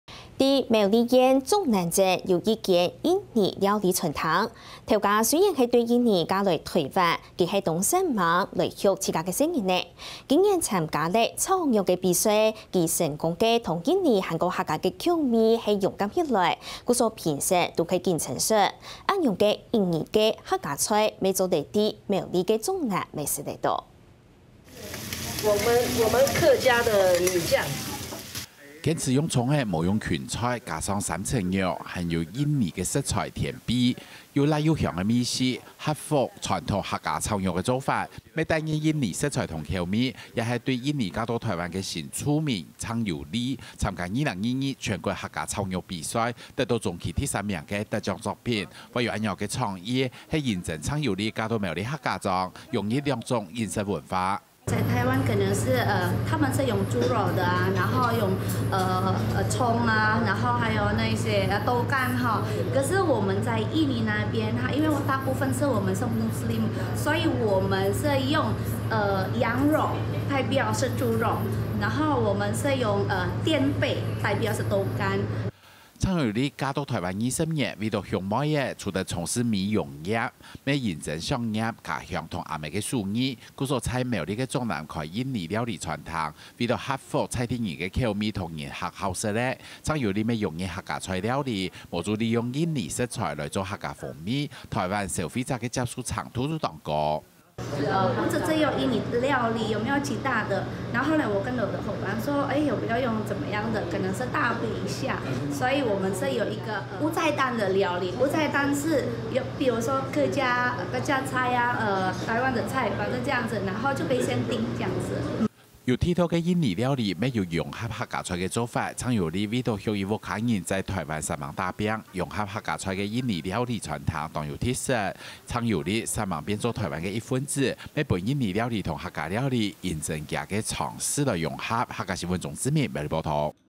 新住民鄭尤莉 靠「印尼客家菜」打拚小有成績【客家新聞20221226】
在苗栗縣竹南鎮，有一家印尼料理餐廳，老闆雖然自印尼嫁到臺灣，但努力生活扶養女兒，今年參加客家小炒比賽，她成功融合印尼與客家口味，讓評審大為驚豔，而這樣的印尼客家菜，也可以在苗栗竹南品嚐。